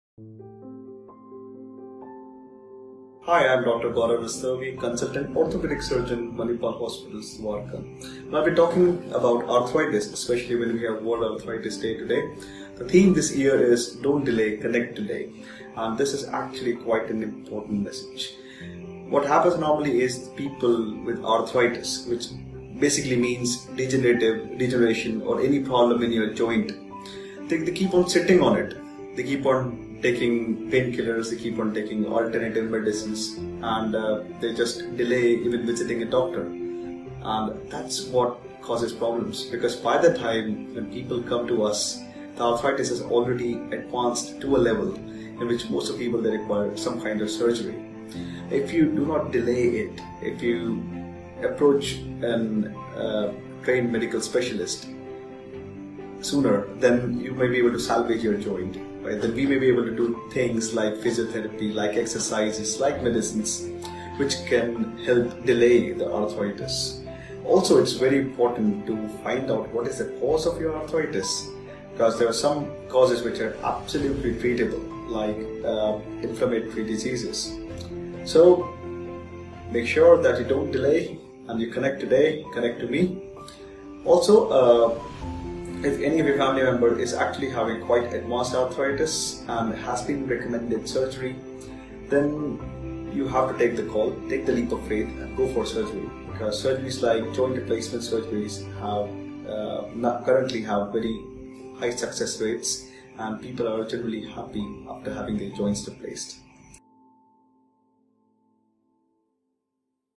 Arthritis | Dr. Gaurav Rastogi | Arthritis Causes | Manipal Hospitals Delhi
Dr. Gaurav Rastogi (Consultant-Orthopedics) tells us on how Arthritis is caused, implications and how a person can take care of himself from the worsening of the disease.